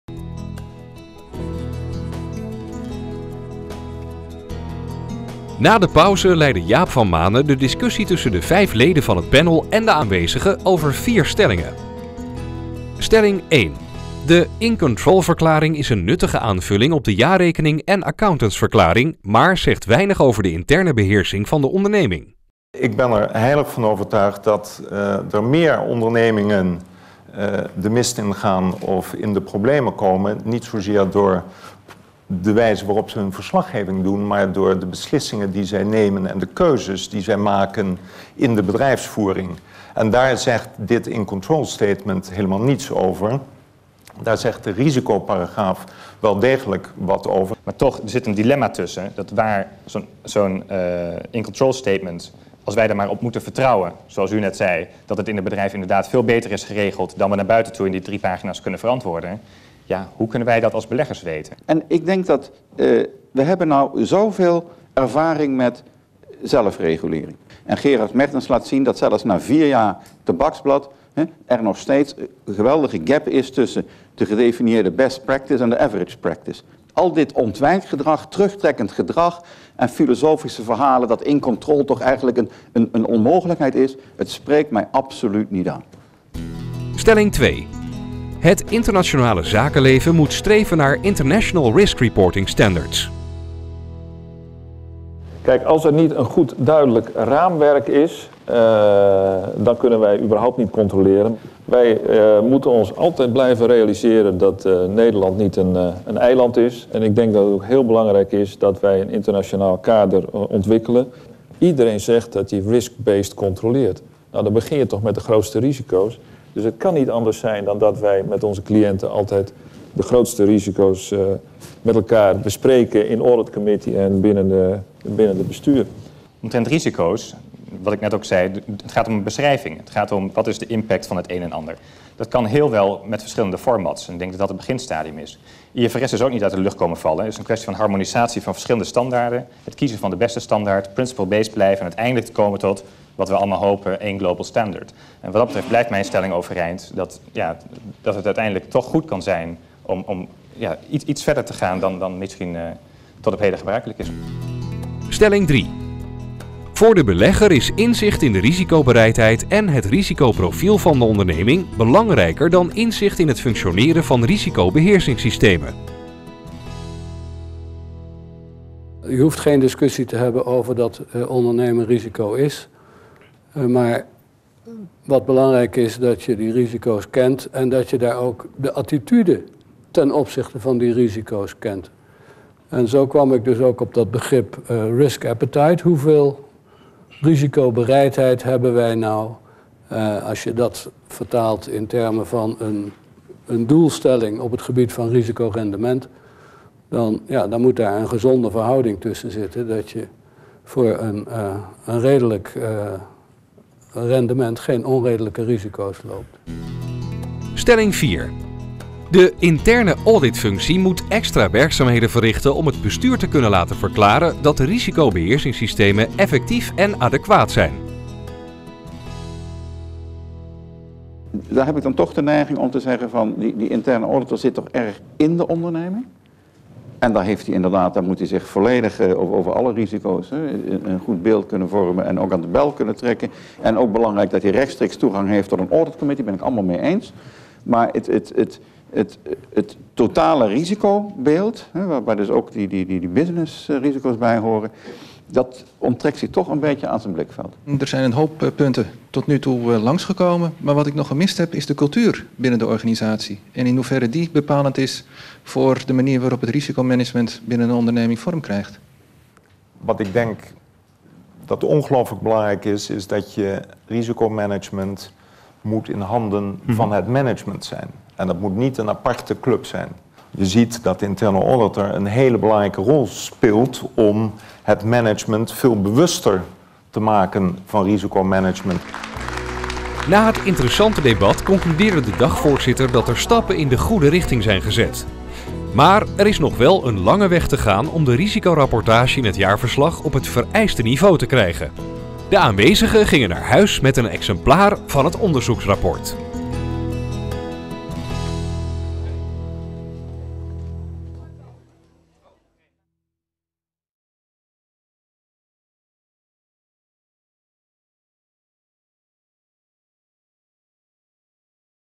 'Inzicht in onzekerheid' (NIVRA/Eumedion) deel 2
Op 24 juni 2008 organiseerden de NIVRA vakgroep FINAD en het corporate governance forum voor institutionele beleggers, Eumedion het seminar 'Inzicht in Onzekerheid'. Beide organisaties hebben een onderzoek laten uitvoeren naar de risico(management)verantwoording in de jaarverslagen-2007 van Nederlandse beursfondsen.